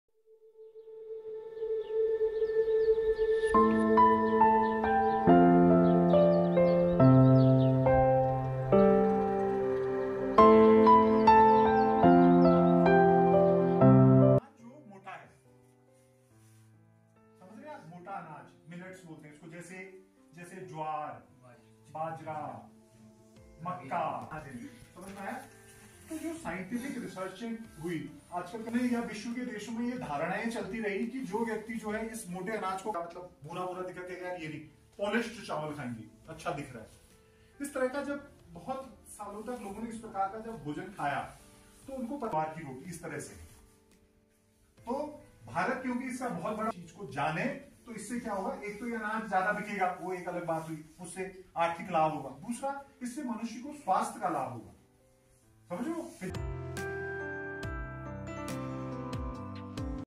International Year Of Millets 2023
In celebration of the International Year of Millets, Colonel Hem Chandra Singh, Principal of JIM and WS Pahalgam, delivered a lecture to  students and staff members of Govt School PM SHRI HS Ahmad and also  students and teachers from Govt High School Kathsoo, actively participated in the event.

The session successfully heightened awareness among students and teachers regarding the significance of incorporating millets into their diets. We are optimistic that the students will carry this valuable message home and contribute to the positive change